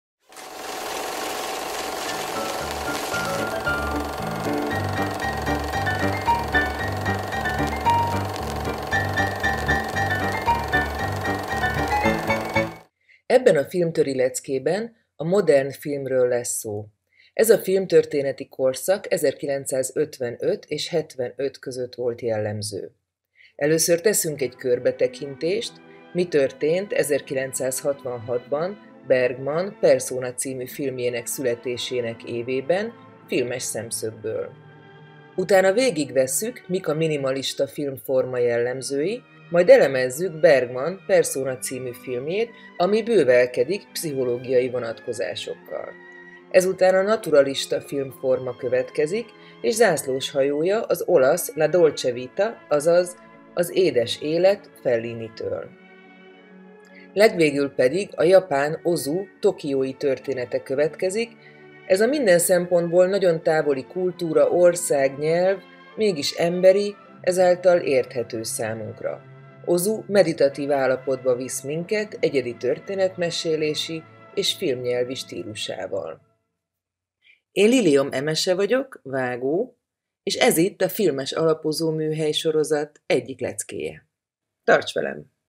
A modern film - Filmes Alapozó Műhely
Ez a tananyag a Filmes Alapozó Műhelysorozat része.
A sorozatban a filmtörténet egyes korszakai és valamilyen mozgókép készítéssel kapcsolatos képességek találhatóak látványos és szórakoztató módon.
A modern film
Ebben a filmtöri leckében a modern filmről lesz szó. Ez a filmtörténeti korszak 1955 és 75 között volt jellemző.
Először teszünk egy körbetekintést, mi történt 1966-ban, Bergman, Persona című filmjének születésének évében, filmes szemszögből.
Utána végigvesszük, mik a Minimalista filmforma jellemzői, majd elemezzük Bergman Persona című filmjét, ami bővelkedik pszichológiai vonatkozásokkal.
Ezután a naturalista filmforma következik, és zászlóshajója, az olasz La Dolce Vita - azaz Édes Élet Fellinitől.
Legvégül a japán Ozu Tokiói története következik, ez a minden szempontból nagyon távoli kultúra, ország, nyelv mégis emberi, ezáltal érthető számunkra. Ozu meditatív állapotba visz minket, egyedi történetmesélési és filmnyelvi stílusával.
A teljes tananyag a Webuni oldalán megvásárolható.